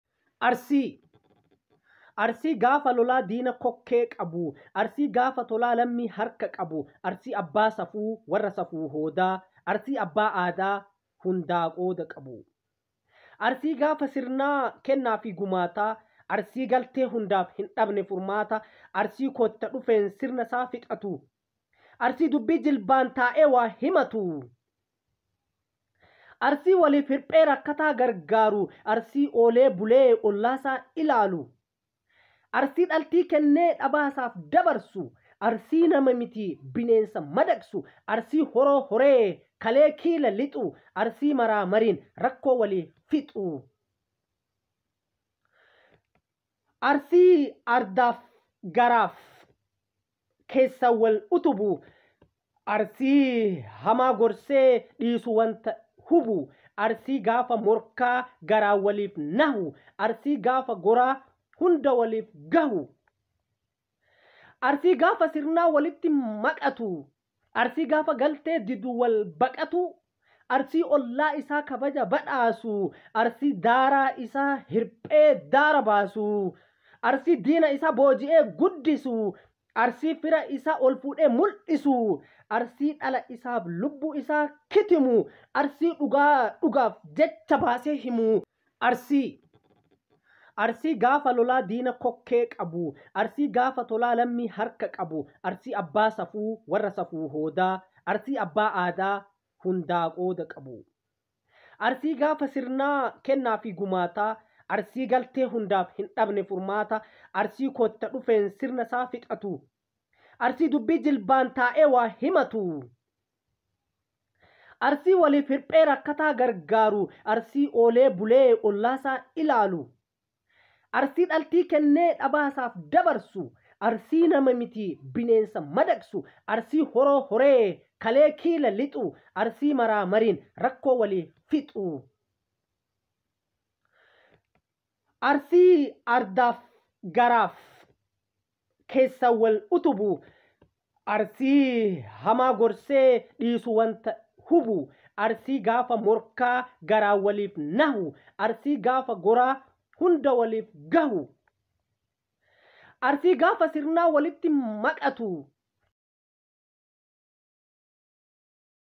Amazing; Mind_Blowing Video About Oromo Arsi
Arsii gaafa lolaa
diina kokkee qabuu
Arsii gaafa tolaa
lammii harka qabuu
Arsii abbaa safuu
warra safuu hoodaa
Arsii abbaa aadaa
hundaa qabu qoodaa

Arsii gaafa sirnaa
kennaa fi gumaataa
Arsii galtee hundaaf
hindhabne furmaataa
Arsii kootta dhufeen
sirna isaa fixatuu
Arsii dubbii jilbaan
taa'ee waa himatuu

Arsii waliif hirphee
rakkataa gargaaruu
Arsii oolee bulee
ollaa isaa laaluu

Arsii dhaltii kennee
dhabaa isaaf dabarsuu
Arsii namaa mitii
bineensaa madaqsuu
Arsii horoo horee
kalee kiila lixuu
Arsii maraa mariin
rakkoo waliif fixuu

Arsii ardaa fi garaaf
keessaan wal utubuu
Arsii hamaa gorsee
dhiisu wanta hubuu
Arsii gaafa morkaa
garaan waliif nahuu
Arsii gaafa goraa
hundaa waliif gahuu

Arsii gaafa sirnaa
walitti maqatuu
Arsii gaafa galtee
didu walbaqatuu
Arsii ollaa isaa
kabaja badhaasuu
Arsii daaraa isaa
hirphee daara baasuu

Arsii diina isaa
booji'ee guddisuu
Arsii fira isaa
olfuudhee muldhisuu
Arsii dhala isaaf
lubbuu isaa kitimuu
Arsii dhugaa dhugaaf
jecha baasee himuu